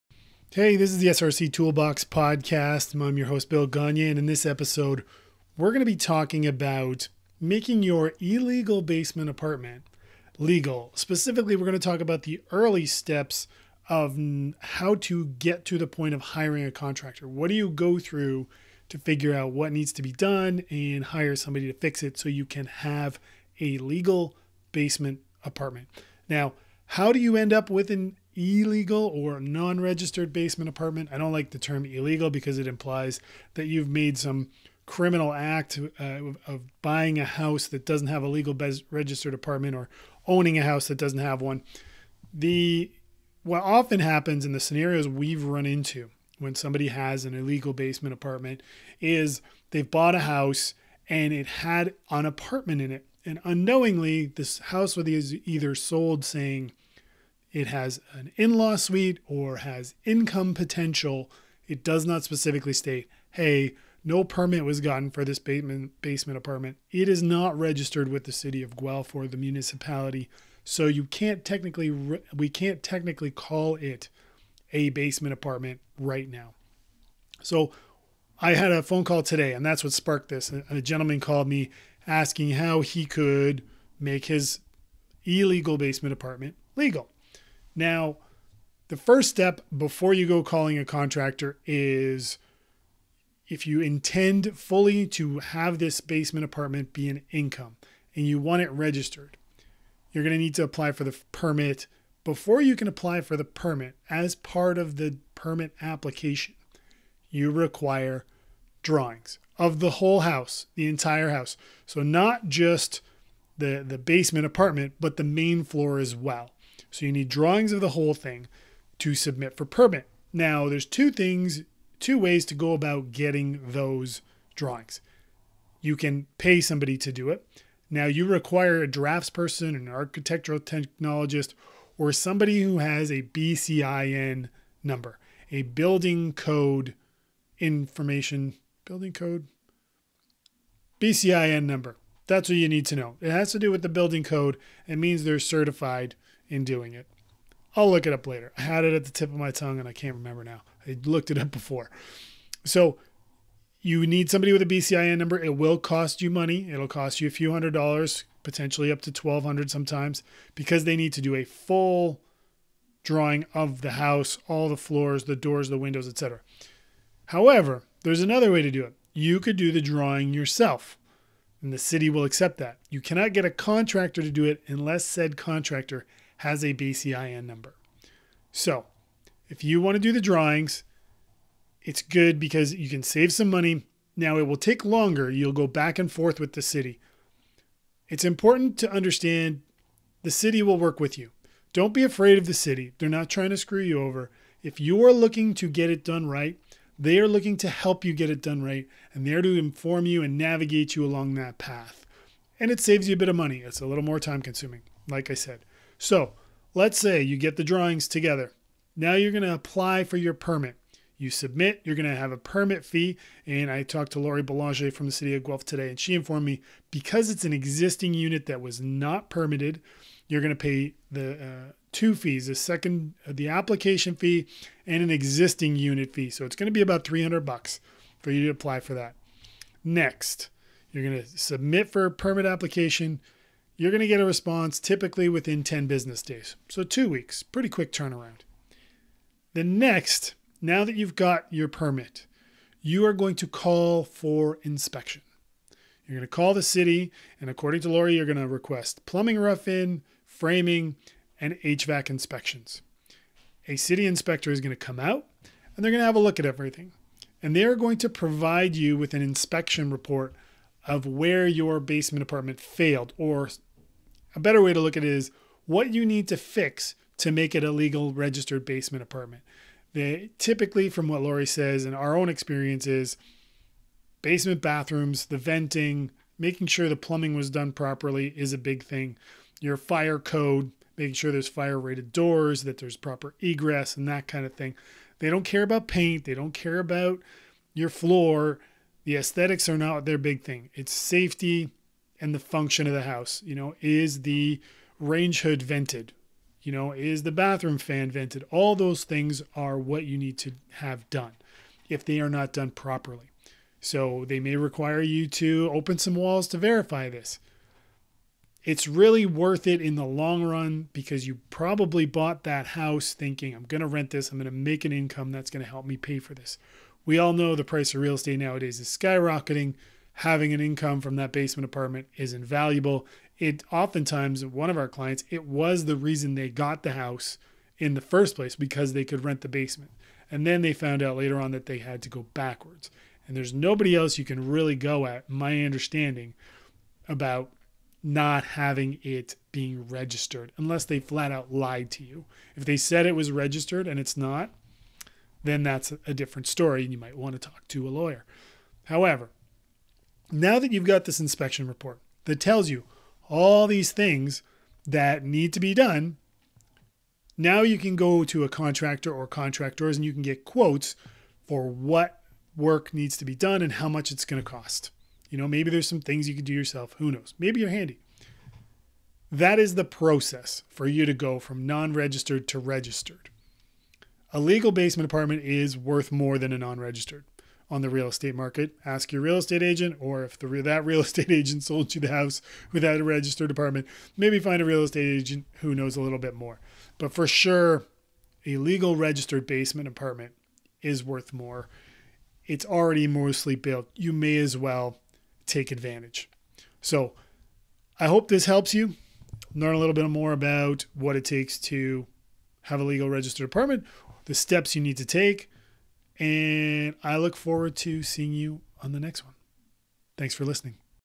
Outlaw Basement ApartmentsL From Illegal to Legal
In this episode of the SRC Toolbox Podcast we discuss a recent conversation with a client about the first steps in taking an illegal/non-registered basement apartment and get it on the path to being a legal/registered basement apartment.

We discuss how these outlaw apartments have come to be and our experience bringing them up to code.